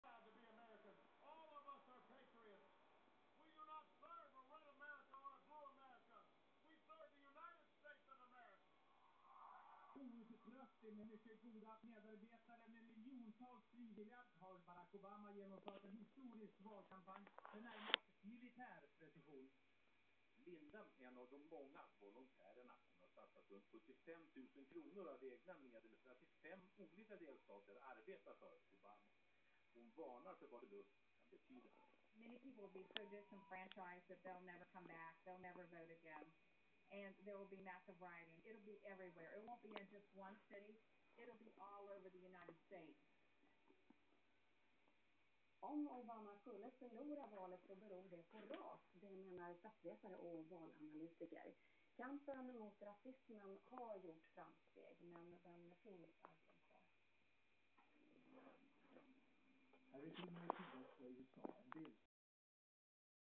Election Subliminal Message 2008 SWEDEN TV4 Stars turned upside-down
It is said upside-down stars like these symbolize the worship of satan/the devil, at least anyone whos Christian or Satanist supposedly knows this.